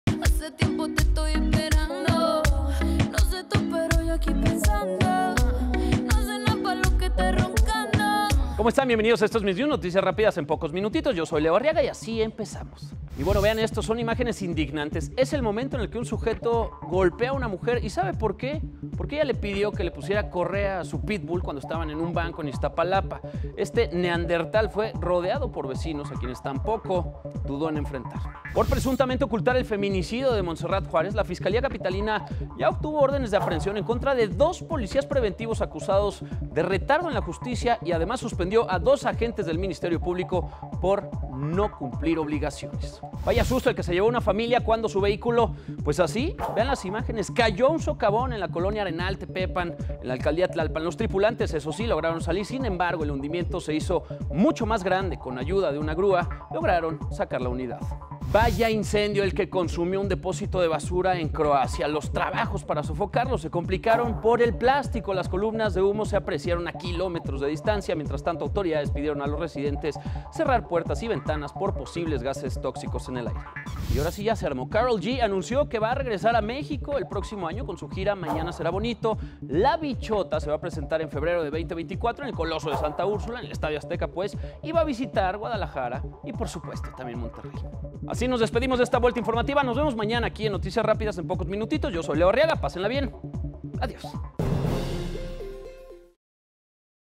MISNIUS | Sujeto golpea a mujer que le pidió que amarrara a su perro
Hoy en MisNius con Leo Arriaga

-Sujeto golpea a mujer que le pidió que amarrara a su perro
-Van contra funcionarios públicos implicados en el Caso Montserrat
-Vehículo cae en socavón ubicado en la alcaldía Tlalpan
-Karol G anuncia fechas sus conciertos en México